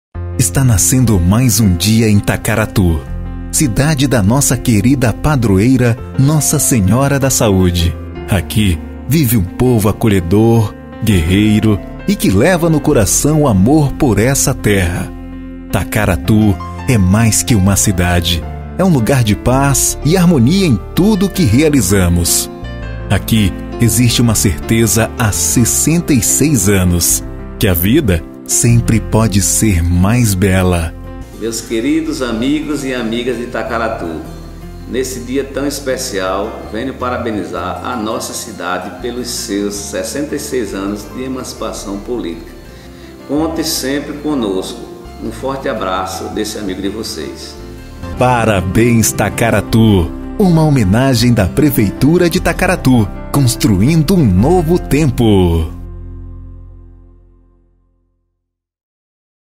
Prefeito de Tacaratu parabeniza o municipio pelos 66 anos de Emancipação Politica por meio de vídeo
A Prefeitura de Tacaratu, no Sertão de Itaparica, parabeniza a população do município pelo aniversário de 66 anos de Emancipação Política. O prefeito José Gerson (PSB) suspendeu as festividades deste ano, por causa da pandemia do coronavírus.

Para mostrar as belezas do município, a prefeitura divulgou um vídeo.